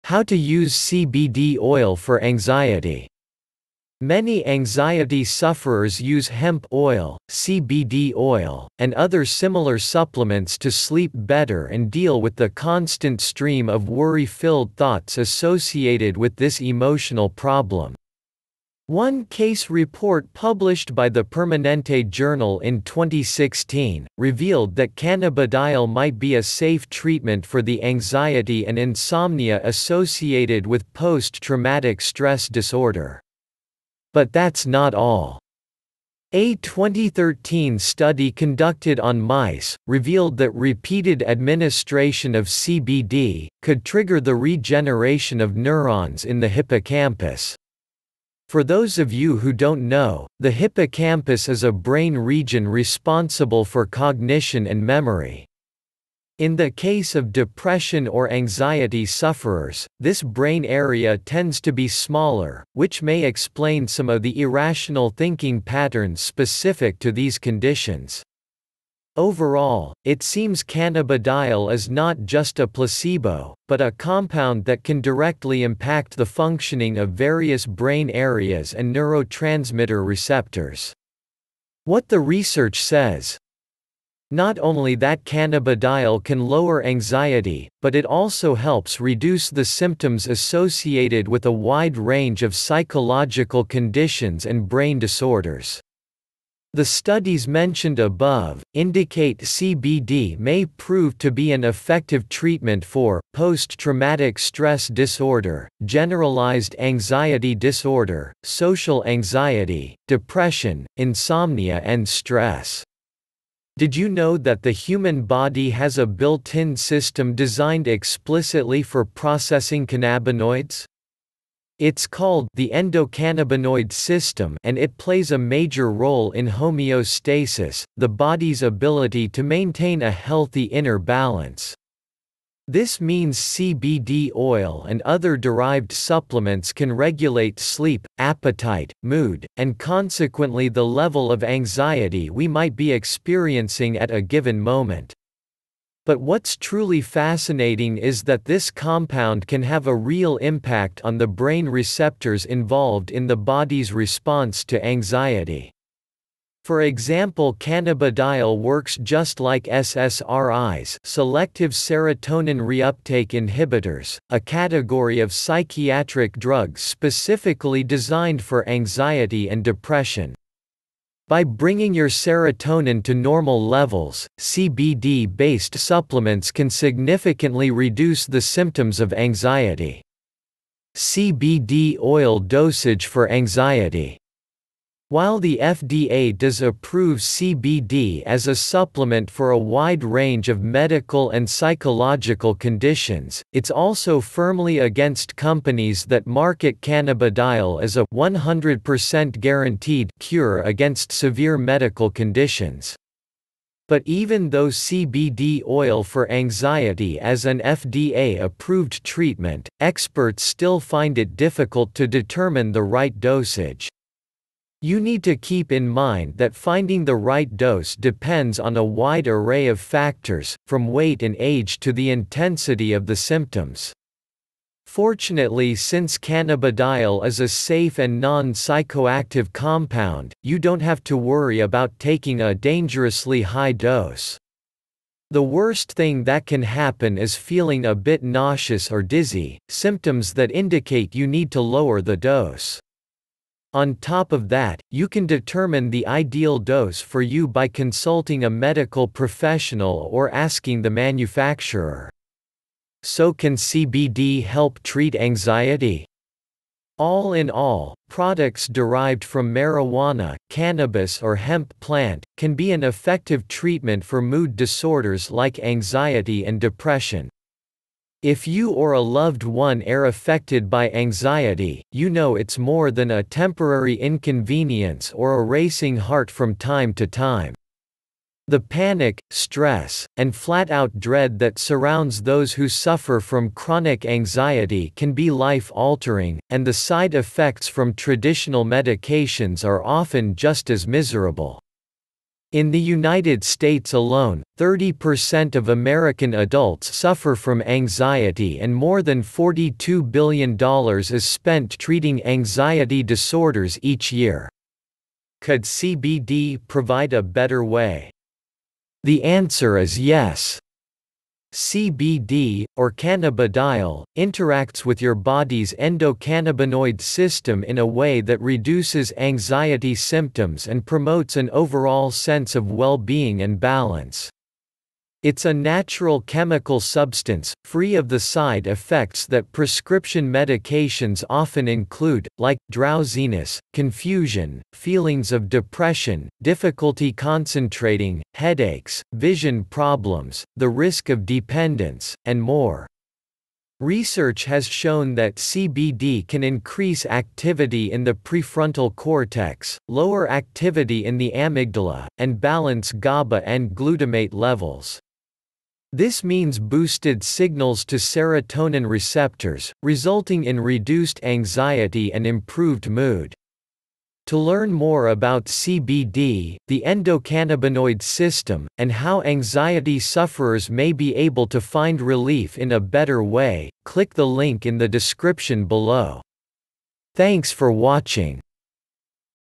CBD Oil For Anxiety - Does it work? (2019)
Top quality best CBD oil i have used.

CBD Oil For Anxiety - Does it work?

If you or a loved one are affected by anxiety, you know it's more than a temporary inconvenience or a racing heart from time to time. The panic, stress, and flat out dread that surrounds those who suffer from chronic anxiety can be life altering, and the side effects from traditional medications are often just as miserable. In the United States alone, 30% of American adults suffer from anxiety and more than $42 billion is spent treating anxiety disorders each year. Could CBD provide a better way? The answer is yes. CBD - or cannabidiol - interacts with your body's endocannabinoid system in a way that reduces anxiety symptoms and promotes an overall sense of well-being and balance. Its a natural chemical substance, free of the side effects that prescription medications often include, like: Drowsiness, confusion, feelings of depression, difficulty concentrating, headaches, vision problems, the risk of dependence, and more. Research has shown that CBD can increase activity in the prefrontal cortex, lower activity in the amygdala, and balance GABA and glutamate levels. This means boosted signals to serotonin receptors, resulting in reduced anxiety and improved mood.

The worst thing that can happen is feeling a bit nauseous or dizzy, symptoms that indicate you need to lower the dose. On top of that, you can determine the ideal dose for you by consulting a medical professional or asking the manufacturer. So can CBD help treat anxiety? All in all, products derived from marijuana, cannabis or hemp plant, can be an effective treatment for mood disorders like anxiety and depression.

To learn more about CBD, the endocannabinoid system, and how anxiety sufferers may be able to find relief in a better way, click the